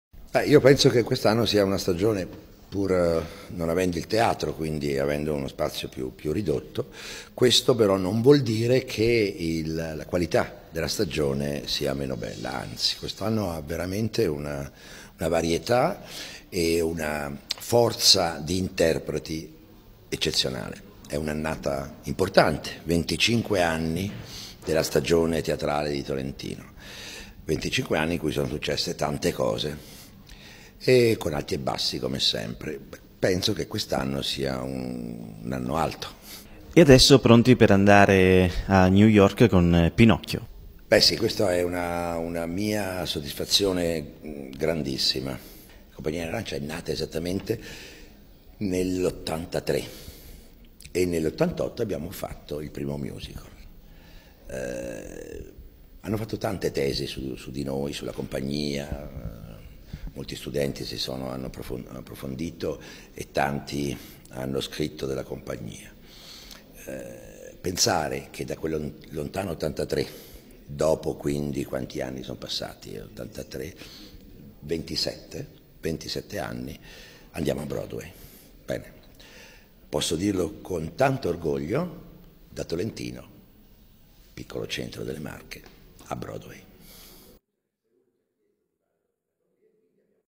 TOLENTINO. TERZA STAGIONE TEATRALE SENZA VACCJ
TOLENTINO. 21 Settembre 2010. Variegato e di qualità il cartellone 2010/2011 della stagione teatrale di Tolentino che senza Vaccaj non si ferma e prosegue per la terza edizione consecutiva sul palco del Cineteatro Don Bosco, segnando il traguardo prestigioso dei  25 anni di stagioni teatrali. Il cartellone, in collaborazione con Compagnia della Rancia e Amat,  prevede otto spettacoli più una rappresentazione fuori abbonamento, da novembre ad aprile, si inizia con un omaggio a "Mattei. Petrolio e Fango".